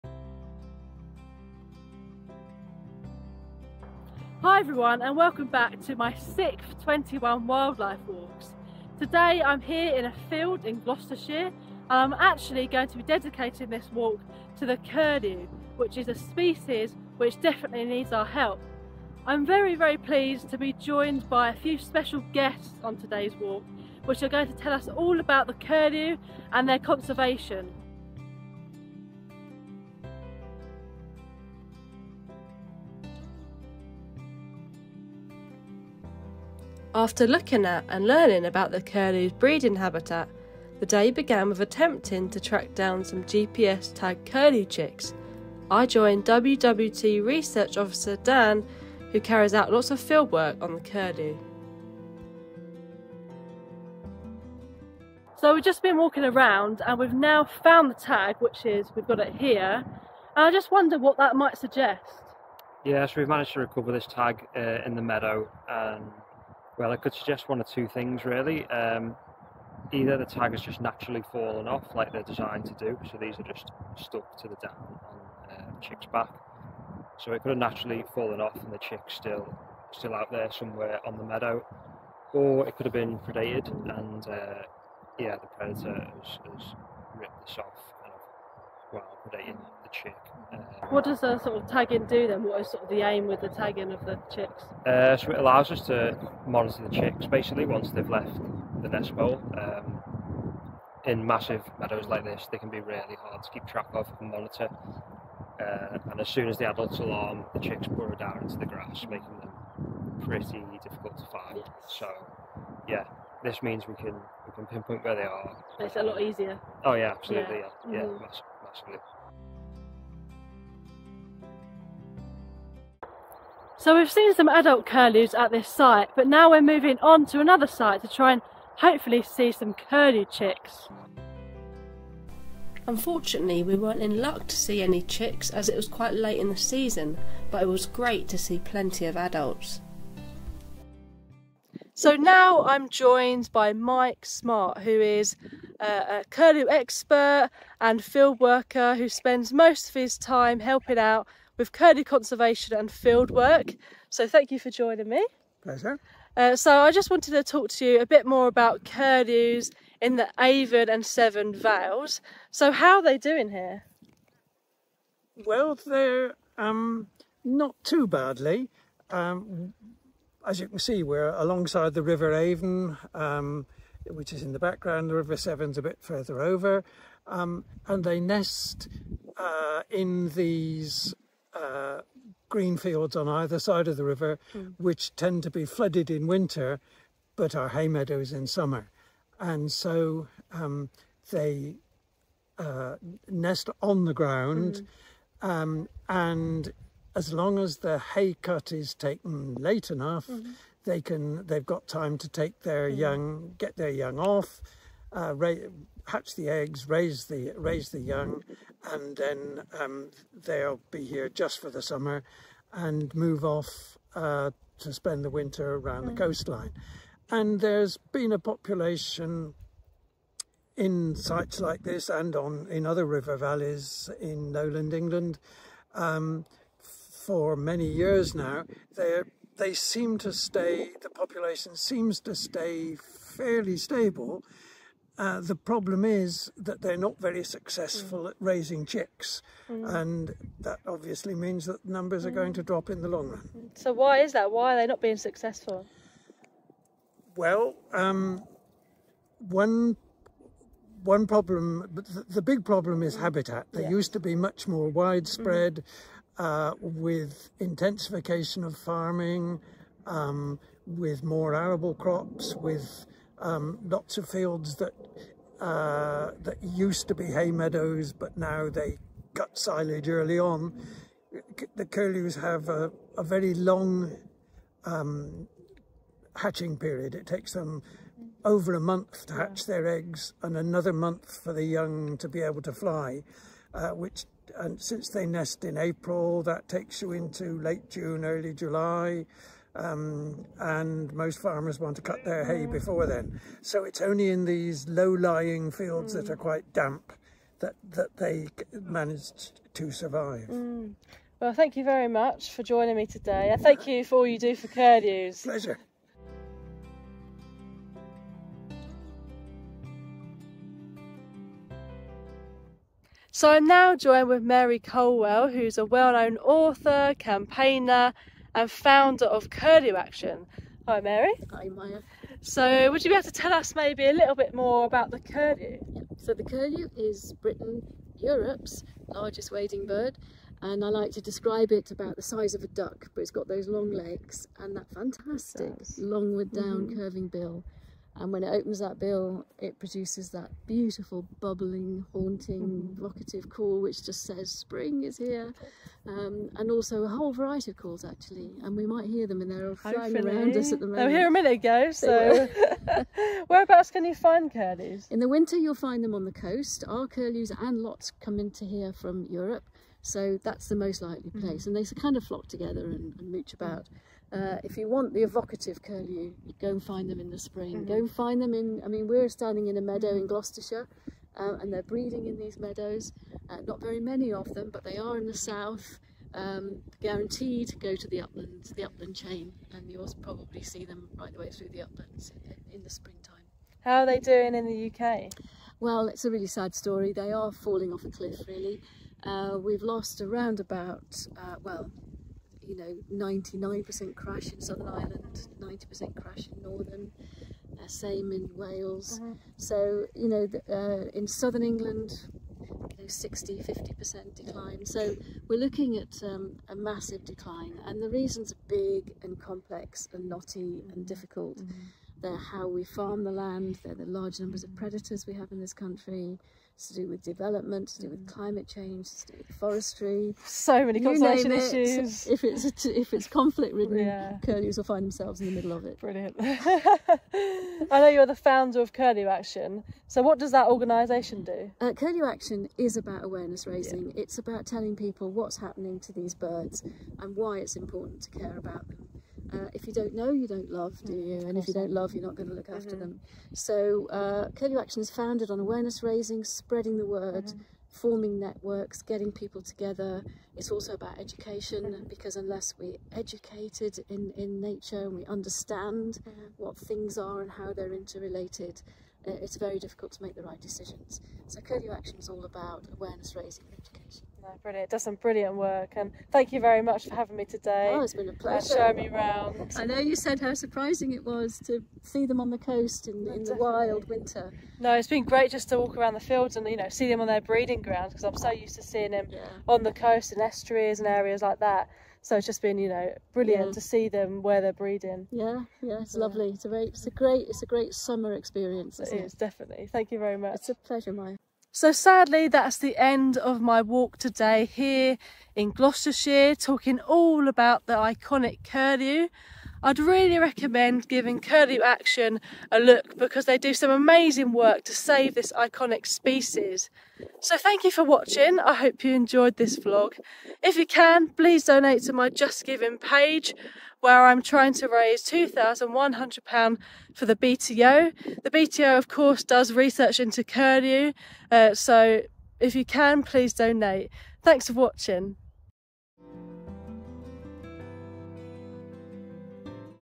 Curlew Conservation Walk - my eighth '21 Wildlife Walks!' with special guests
This is the eighth of my ‘21 wildlife walks,’ where I’m going on and vlogging 21 different wildlife walks across the UK before my 21st birthday in September with the aim to raise £2100 for the British Trust for Ornithology.

In this vlog I visit the Severn and Avon Vales to learn more about the Eurasian Curlew and it's conservation. Thanks to Mary Colwell, Mike Smart, and Daniel Gornall for joining me.

Keep your eyes pealed on my YouTube and social media for new vlogs and updates!